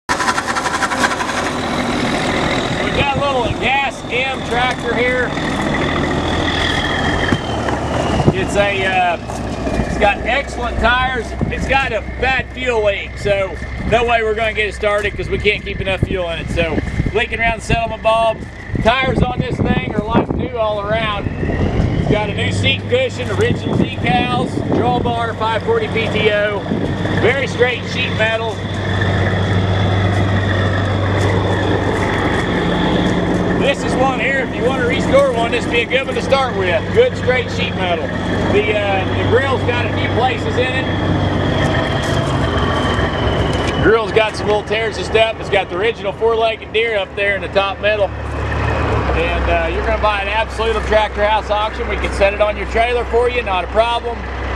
JOHN DEERE M For Sale
John Deere M Tractor  -Gas, 540 PTO, Fuel Leak, New Seat Cushion, Like New Tires, Ding In Grill  *See Pics & Video For More Details *Sells Absolute! ~Please Read Terms & Conditions Before Bidding*